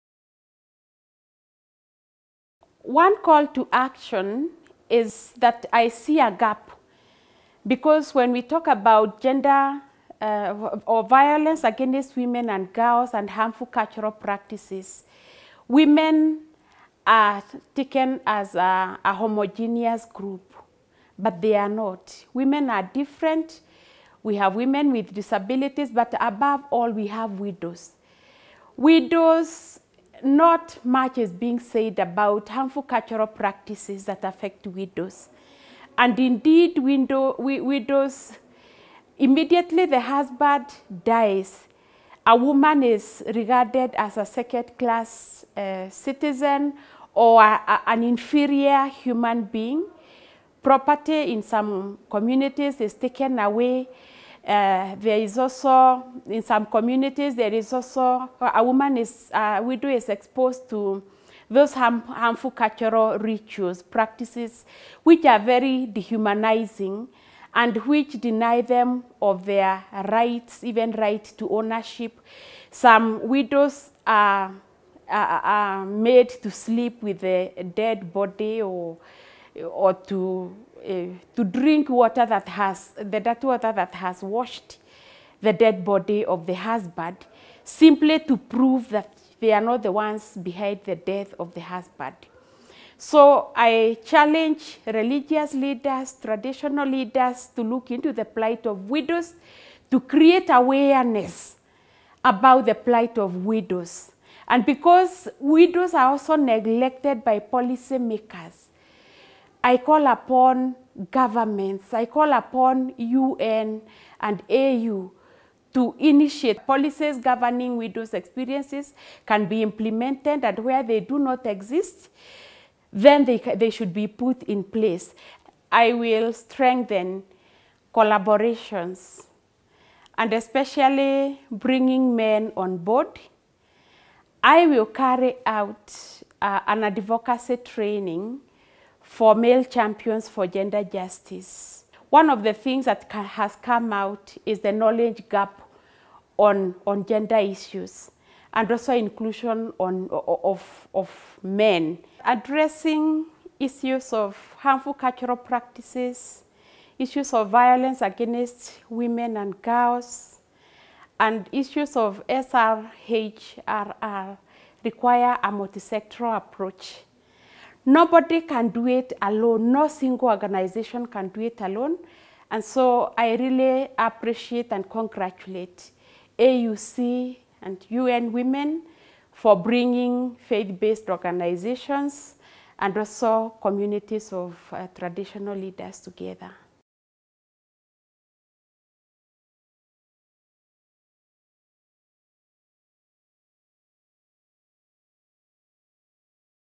Reverend Dr. Lydia Mwaniki on harmful practices and how they affect women differently
Strengthening the role of Traditional and Religious Leaders in ending violence against Women & Girls.

Traditional, religious and civil society leaders of Africa gathered on 19 October in Addis Ababa to confirm their commitment in joining hands with the African Union Commission, the United Nations and the Spotlight Initiative and taking bold action to end child marriage, female genital mutilation (FGM) and other harmful practices.